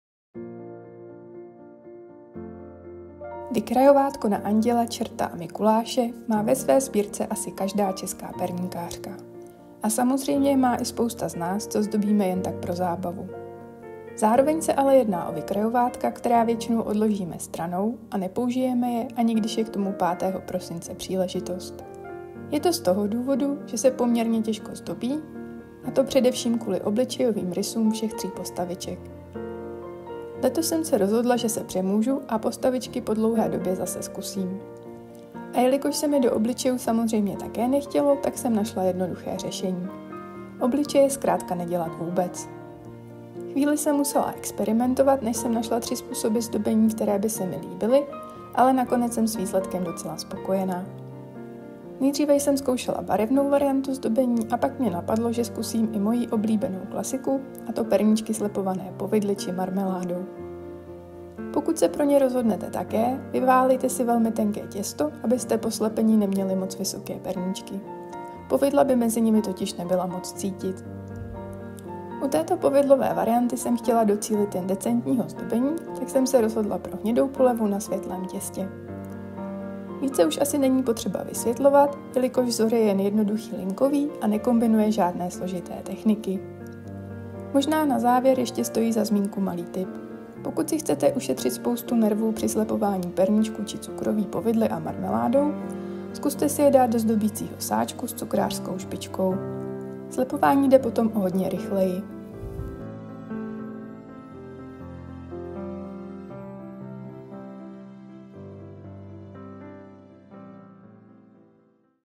Mikulášské perníčky
💙 Vykrajovátko na anděla, čerta a Mikuláš má ve své sbírce asi každá česká perníkářka. A samozřejmě je má i spousta z nás, co zdobíme jen tak pro zábavu. Zároveň se ale jedná o vykrajovátka, které většinou odložíme stranou a nepoužijeme, ani když je k tomu 5. prosince příležitost.

Terka 💙